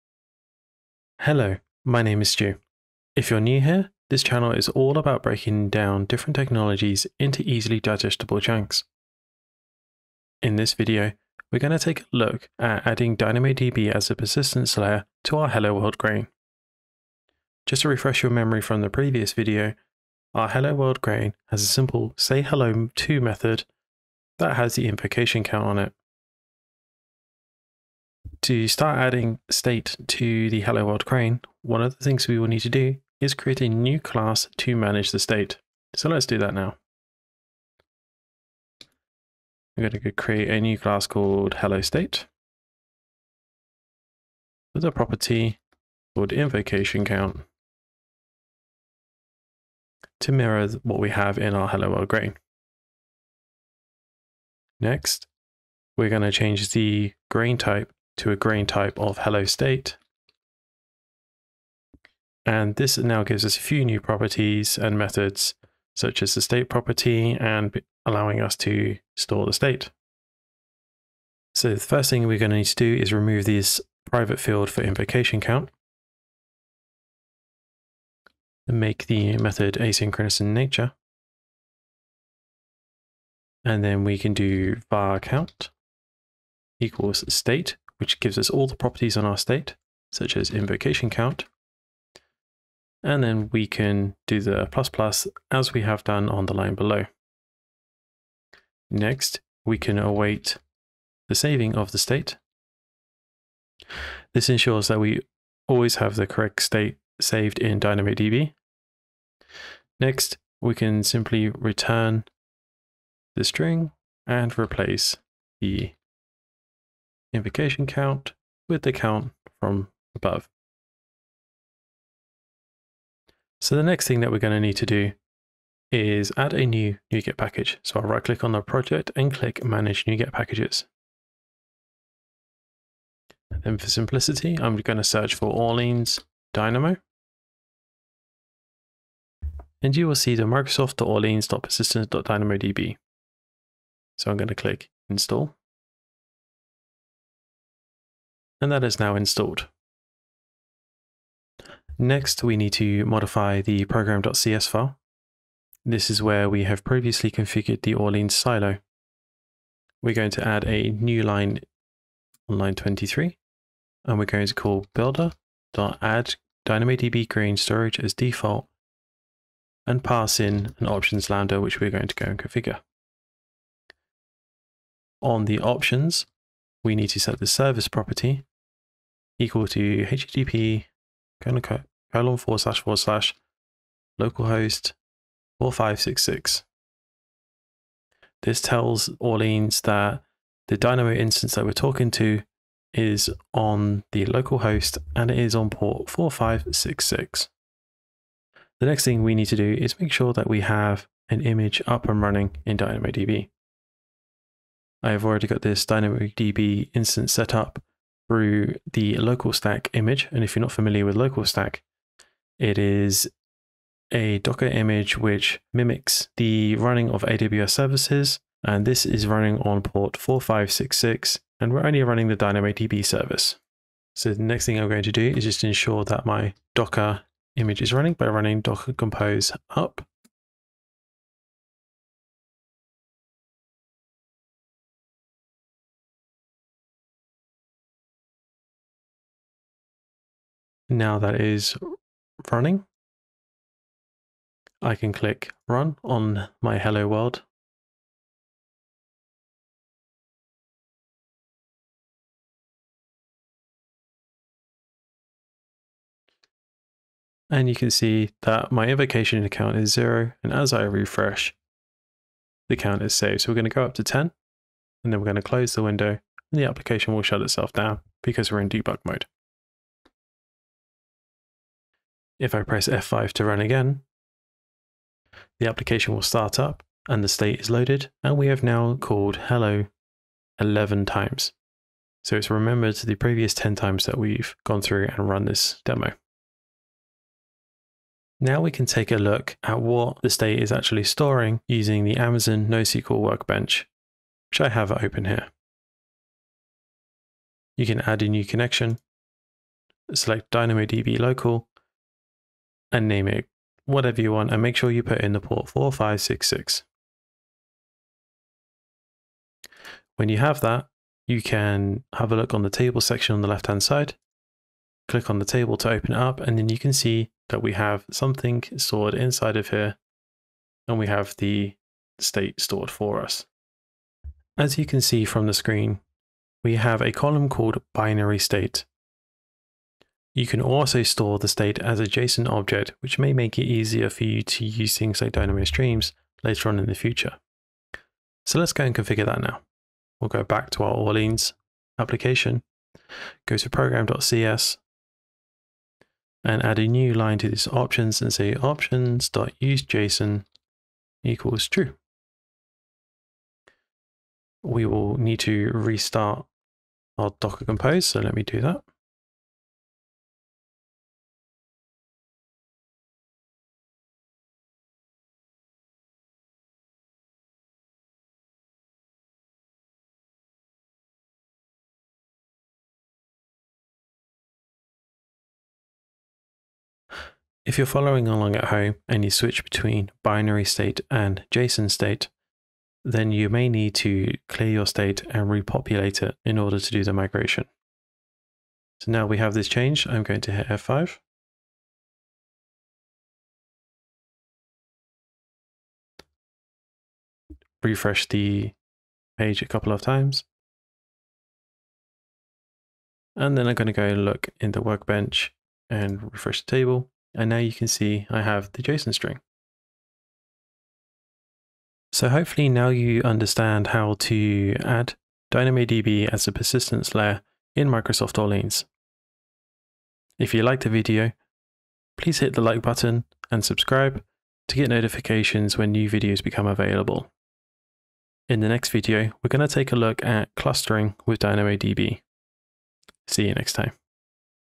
Persisting Microsoft Orleans Grain State With DynamoDB
Following on from the previous video, you'll learn how to setup grain persistence with DynamoDB local. This video is part of the Running Microsoft Orleans in AWS series.

0:00 - The Basics of Microsoft Orleans Grain State with DynamoDB
0:27 - Configuring the state class
1:58 - Persisting the state in DynamoDB
3:18 - Setting up local testing with Docker Compose
4:45 - View the state using the NoSQL Workbench